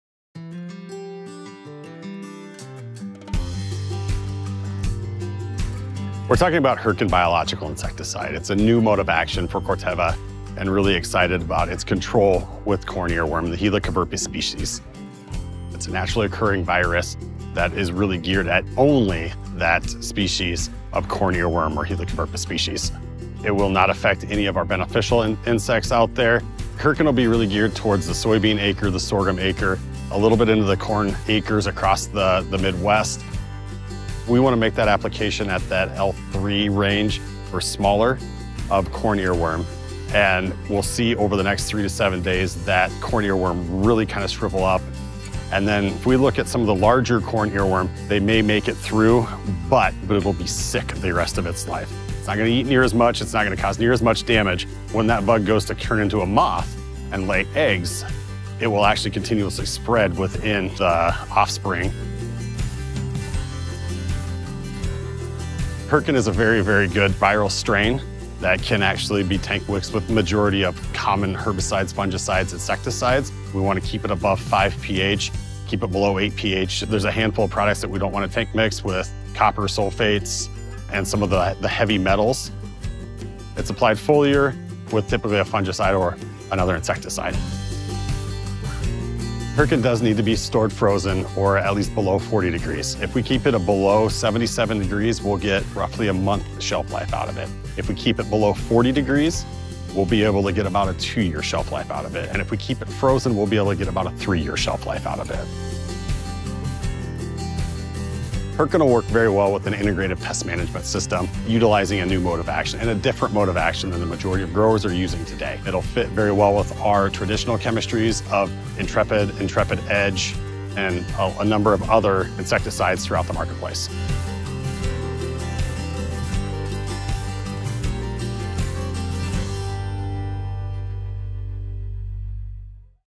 A New Mode of Action With Hearken® Biological Insecticide
Hearken® biological insecticide can provide a new mode of action to control pests like corn earworm, tobacco budworm, and Old World bollworm. See how the solution works and how you can add it to your row crop protection program.